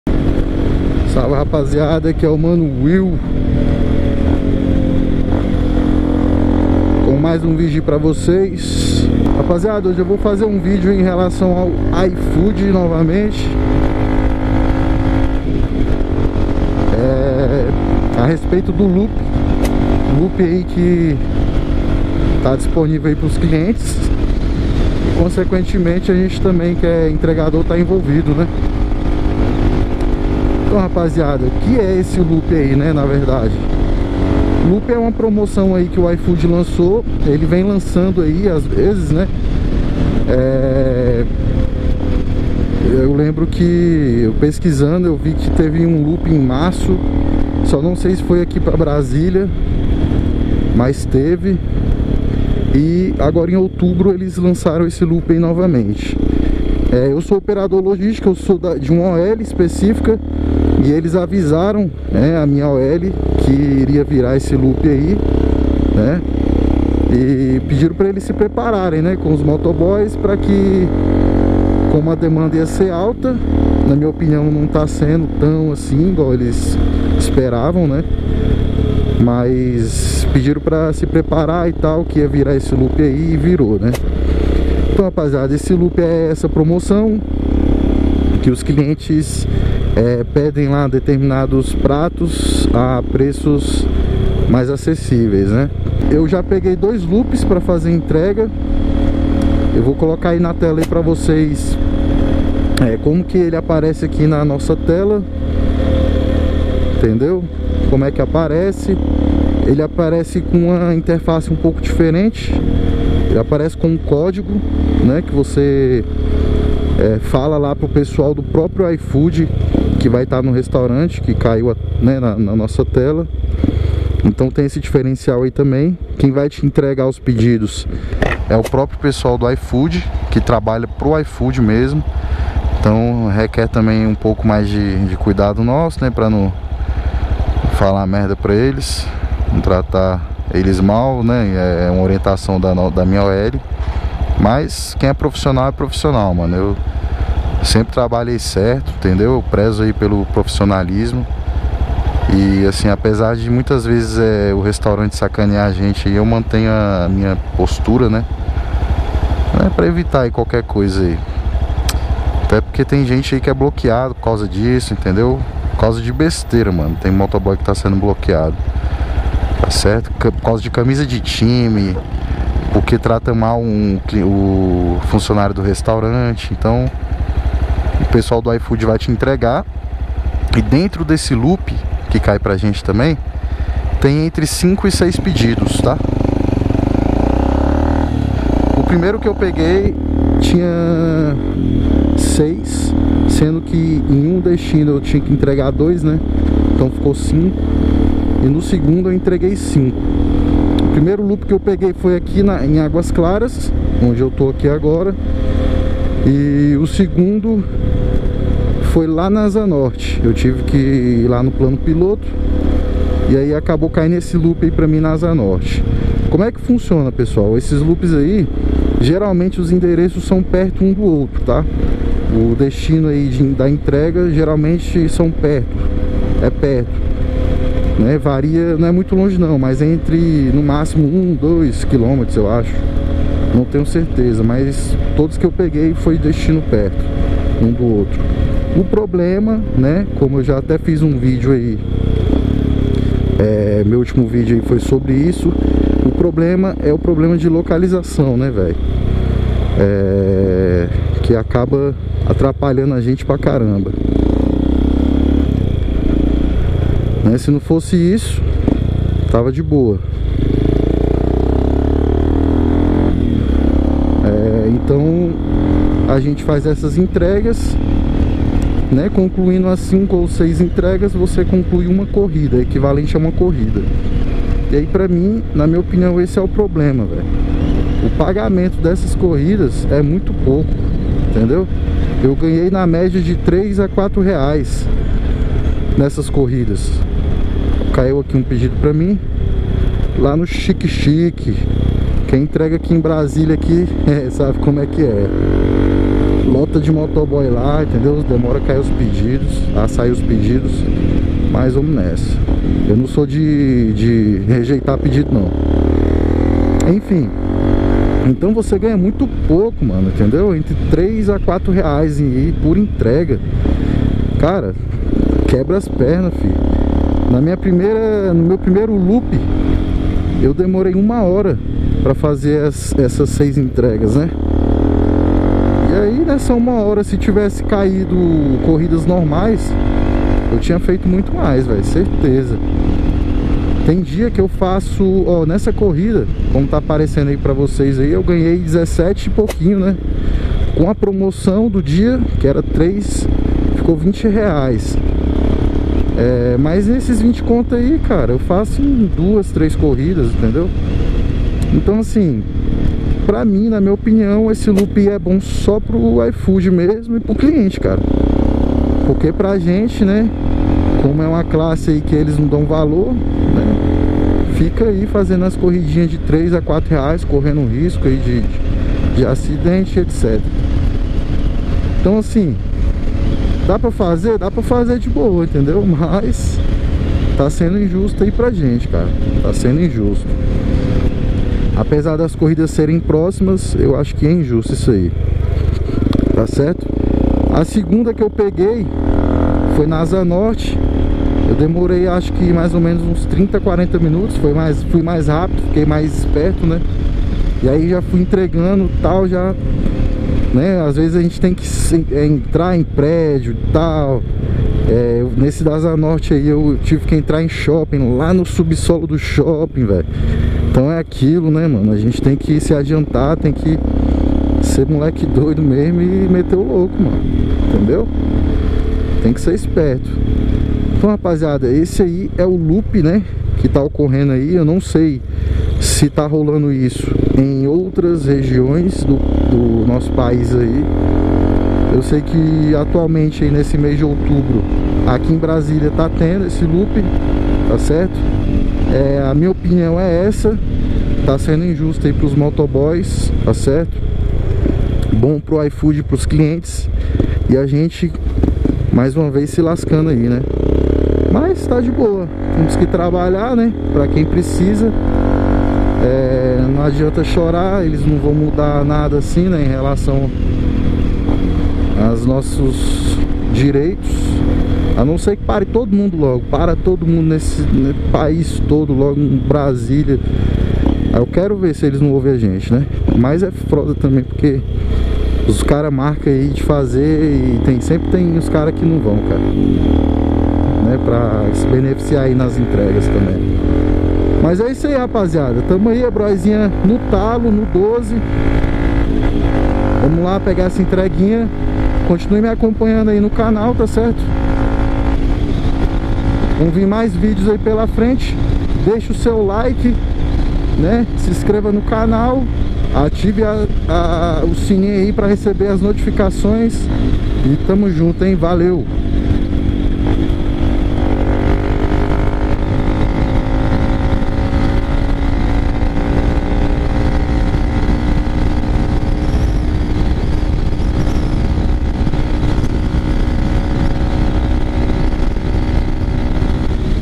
*LOOP IFOOD* COMO FUNCIONA? O BOY SEMPRE LEVA A PIOR
Como funciona o Loop Ifood? Vale a pena para os entregadores? Assista e deixe seu comentário.